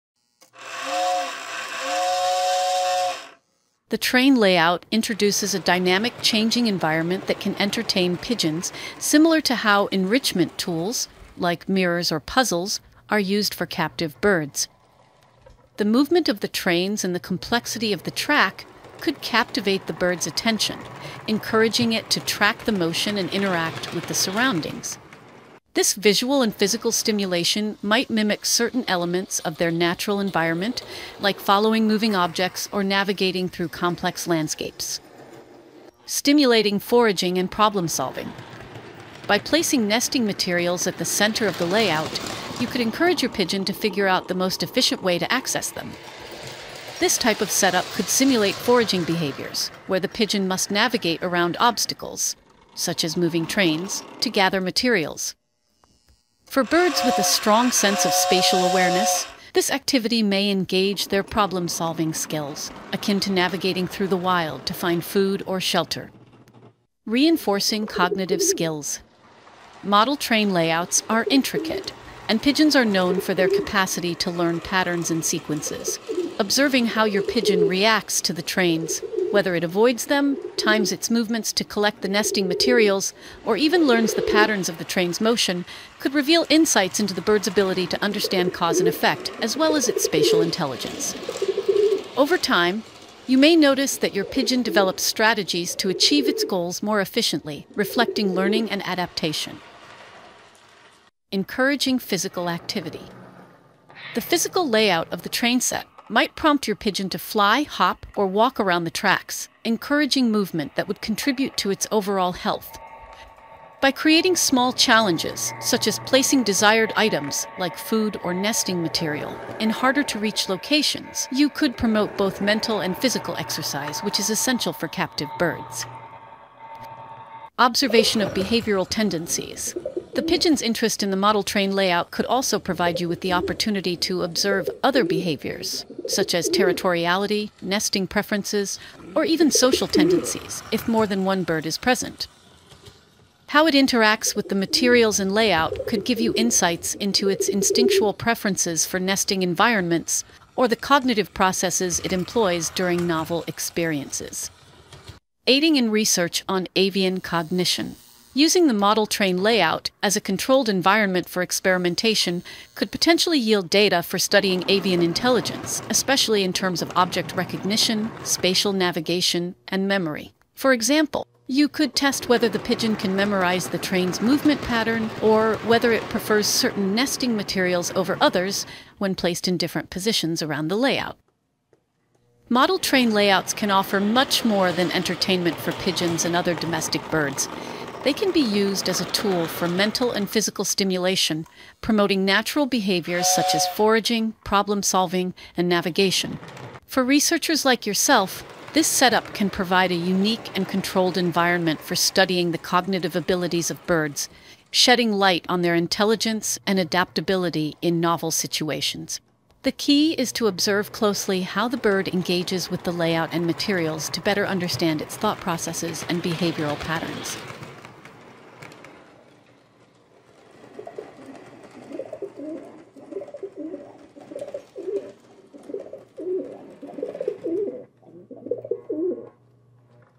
Pigeons and Model Railroads: an enrichment environment
As a researcher studying pigeon intelligence, the interaction between your pet pigeon and the Lionel 0-gauge train layout opens up intriguing possibilities for exploring avian behavior and cognition. Birds, including pigeons, are known for their curiosity and problem-solving abilities, and introducing them to novel environments can stimulate these traits. In this scenario, the model train layout may serve multiple functions in terms of enrichment, mental stimulation, and behavioral expression for domestic birds.

Ensuring the bird’s safety is crucial, as the layout and its components may pose certain hazards. Always supervise your pigeon when it is interacting with the model train layout. Birds are naturally curious, and without supervision, they might engage in potentially harmful activities such as pecking at small parts, chewing on wires, or getting too close to moving trains. Supervision allows you to intervene quickly if the bird encounters any risks.

Narrated by Juliet Hanlon.